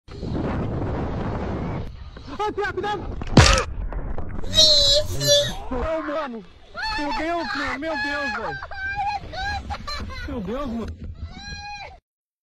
BMX crash into dude on dirt jump
BMX guy crashes into kid just standing on a dirt jump. Kid should have no been standing in the path but DAMN!
Want to grow your channel? Click this link to grow your channel and start making money from youtube TODAY